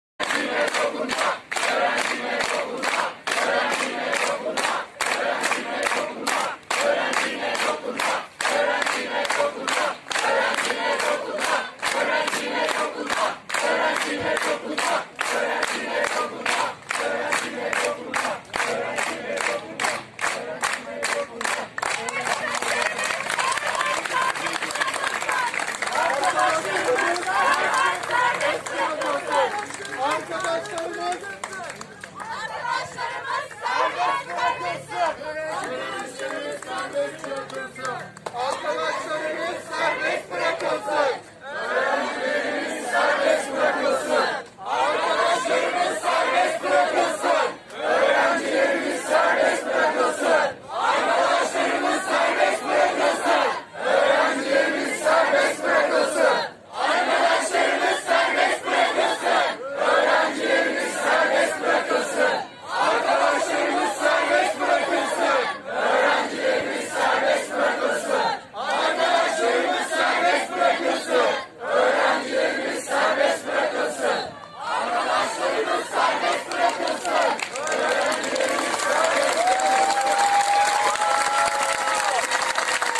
Boğaziçi Üniversitesi protestolar devam ediyor
Kayyum rektör atamasından sonra Öğrenciler ve hocalar birlikte gözaltındaki arkadaşlarının/öğrencilerinin serbest bırakılmasını talep ediyor.